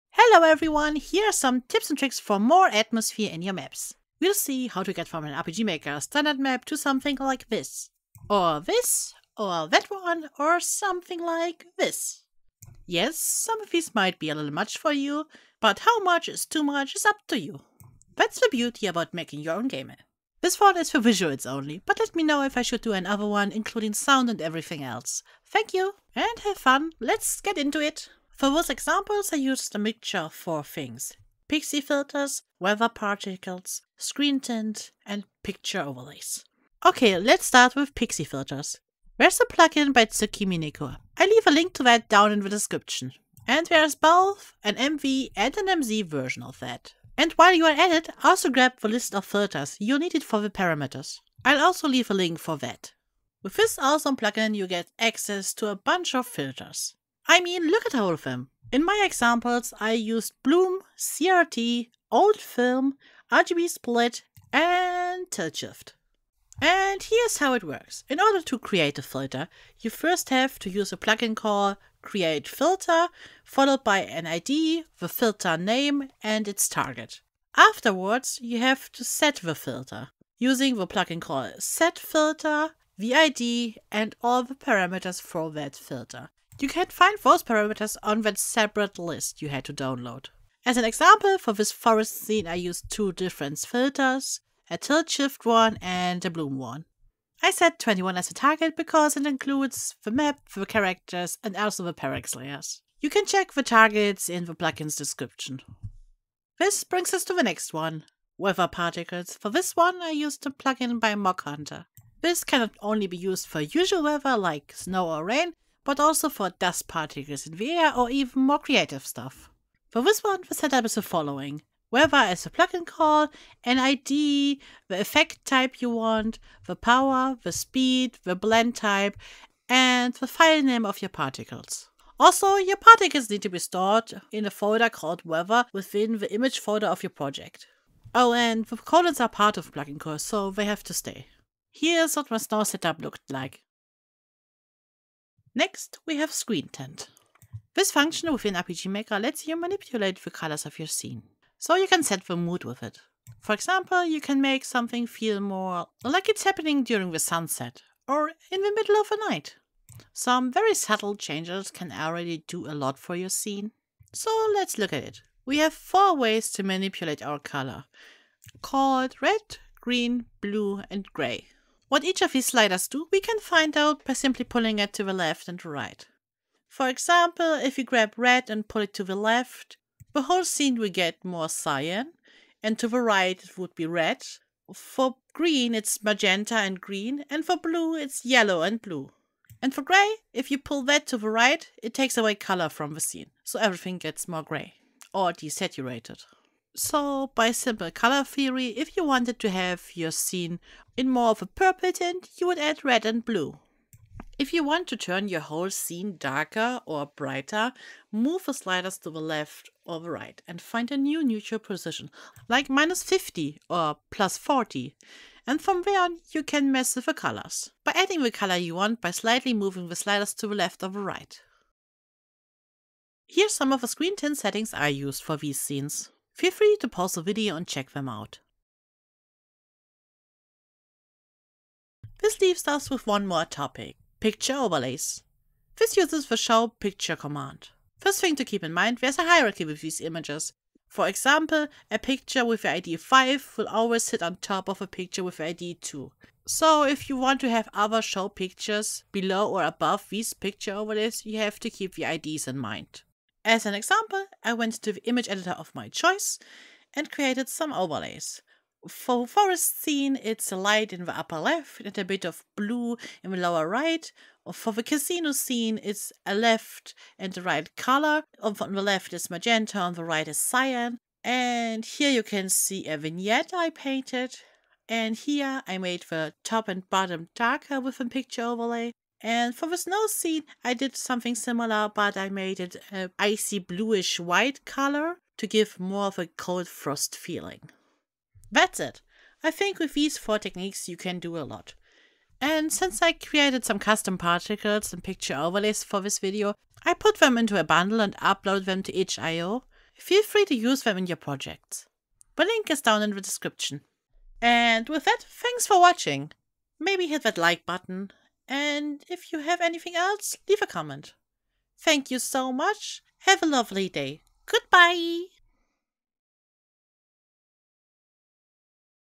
Tips to improve your games graphics. RPG Maker MV & MZ Tutorial
Here are 4 quick tips and tricks for more atmosphere in your RPGMaker games! Improve your graphics with these plugins and functions: Pixi filters aka shaders, particle weather effects, screen tint and picture overlays! These are easy ways to give your game's look an extra boost. Get more out of your game, even if you using "only" the standard tiles aka RTP!

Timestamps
1. Intro 0:00-0:27
2. Content Overview 0:28-0:35
3. Pixi Filters/Shaders 0:36-1:46
4. Weather Particles 1:47-2:24
5. Screen Tint 2:25-3:51
6. Picture Overlays 3:52-4:47
7. Assets and Outro 4:48-5:20